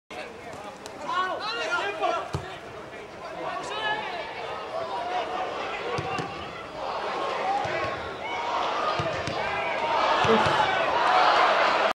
Jack Brady Denies Kurtis Byrne With 3 Successive Saves
Jack Brady blocks three efforts from Kurtis Byrne in the pre-season friendly game at Richmond Park.